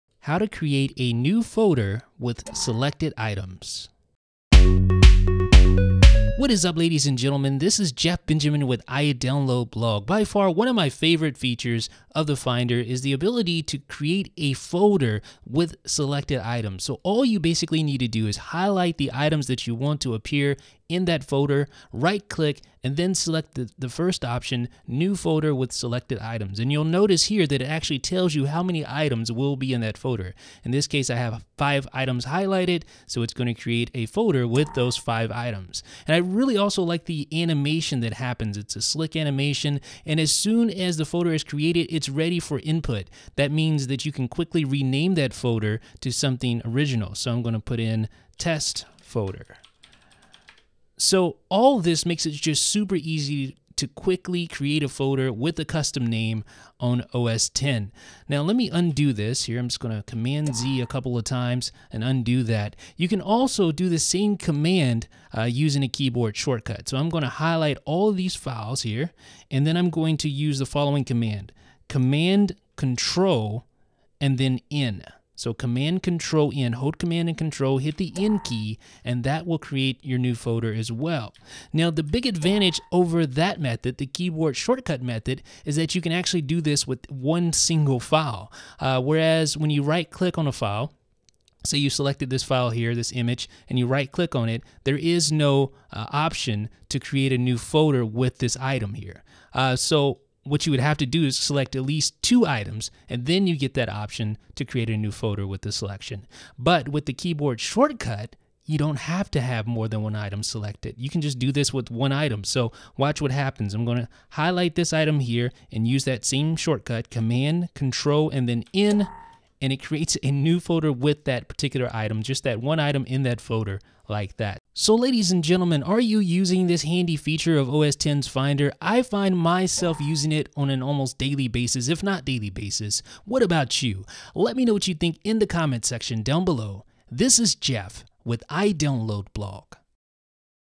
Mac Tip: How to create a new folder containing selected items
Description: This video shows you several ways to quickly create new folders containing highlighted items on OS X.

About iDB